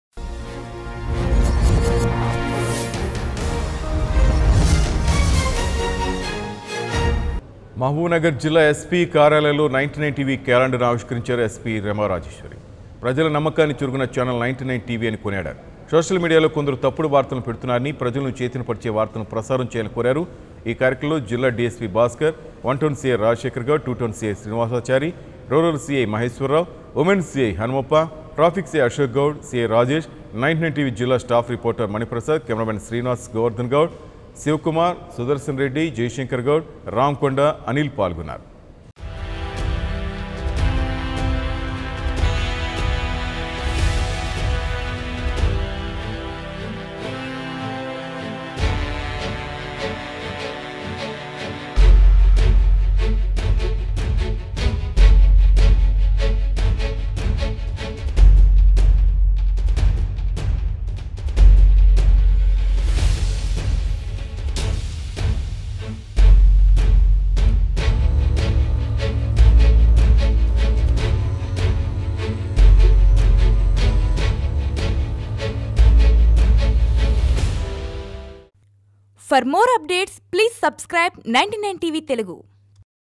99TV New Year Calendar Launched by SP Rema Rajeshwari at Mahabubnagar | 99TV Telugu
99TV New Year Calendar Launched by SP Rema Rajeshwari at Mahabubnagar | 99TV Telugu